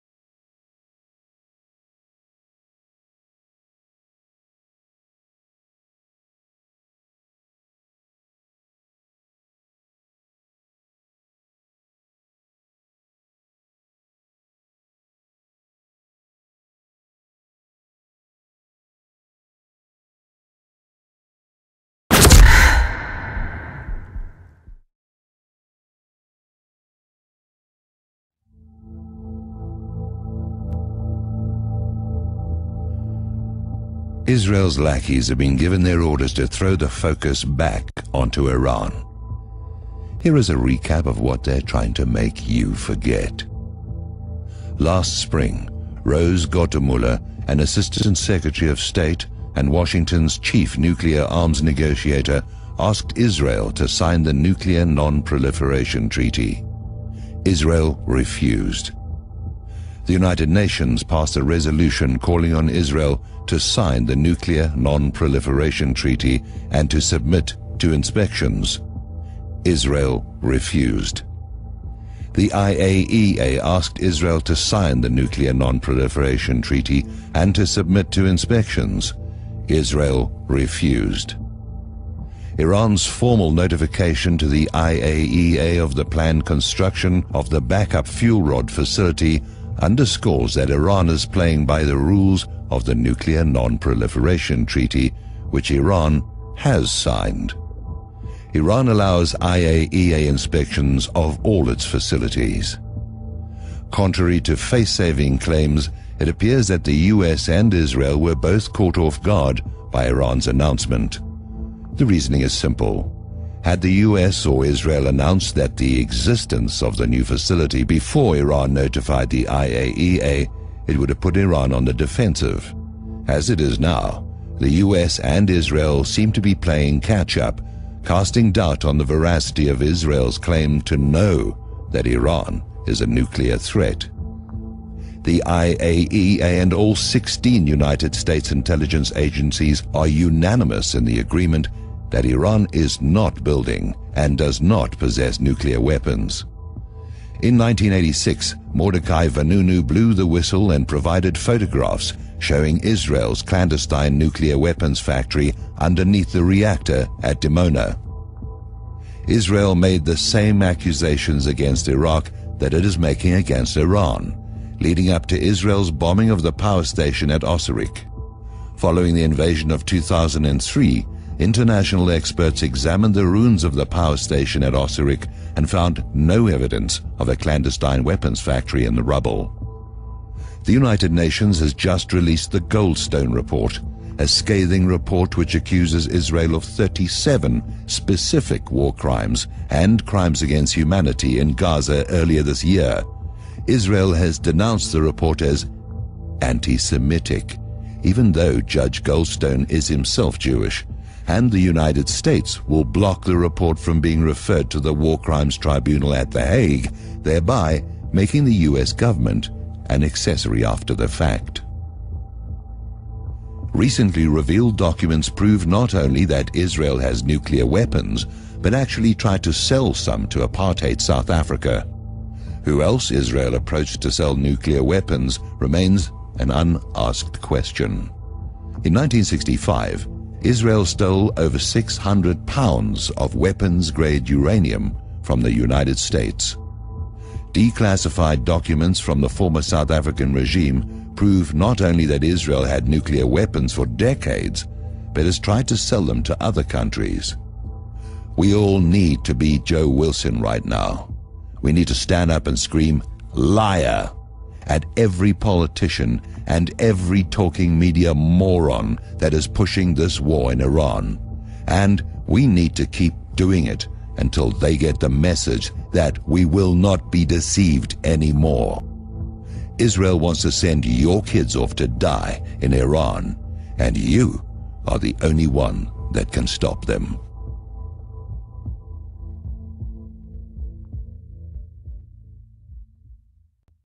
IAEA 2011 - Warum sollte ausgerechnet der Iran angegriffen werden?
Eure Kinder müssen für uns sterben - Warum sollte ausgerechnet der Iran angegriffen werden?

Quellen/Sources:

Washington negotiator calls on Israel to sign nuclear Non Proliferation Treaty

Revealed: how Israel offered to sell South Africa nuclear weapons